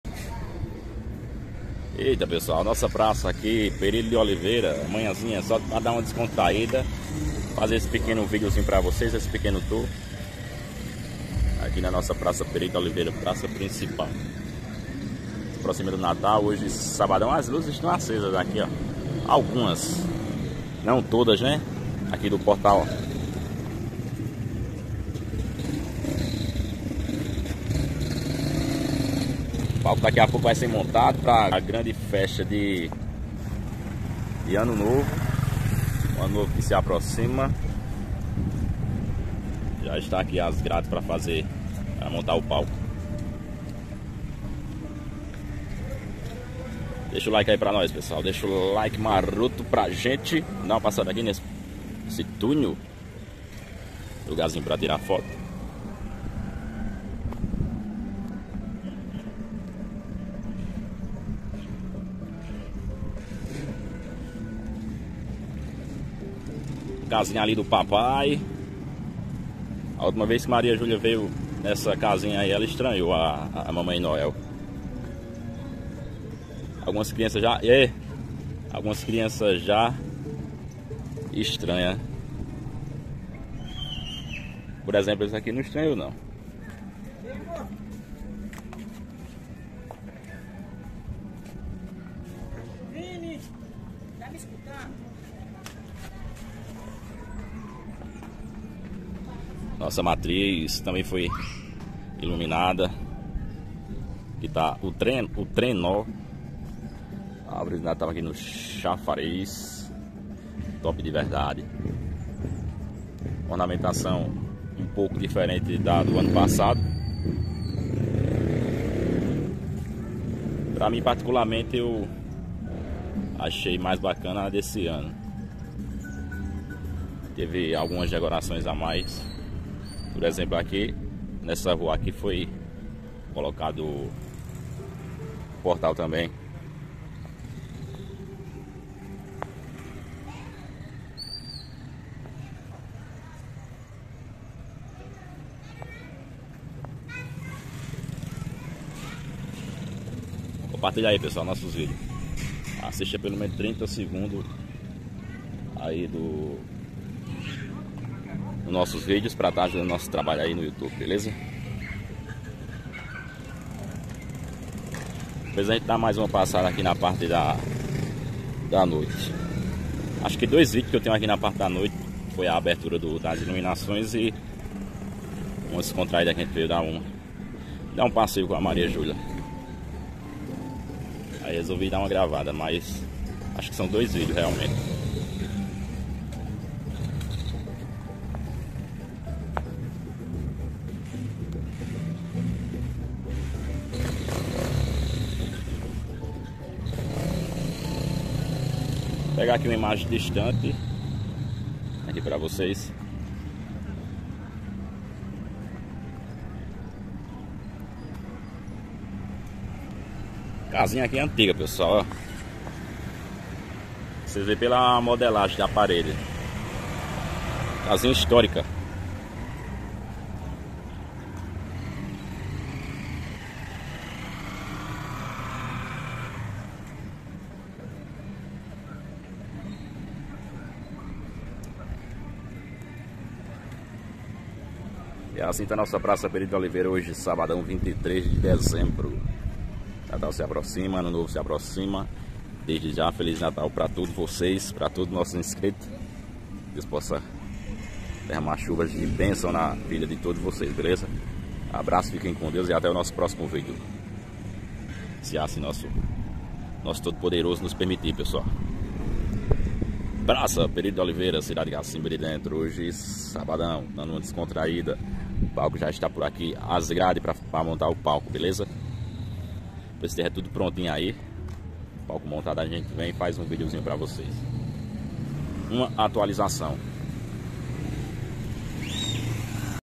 TOUR PELA PRAÇA PERILO DE OLIVEIRA DE CACIMBA DE DENTRO PB - VEJA COMO FOI A ORNAMENTAÇÃO ESSE ANO
Praça Perilo de Oliveira em Cacimba de Dentro PB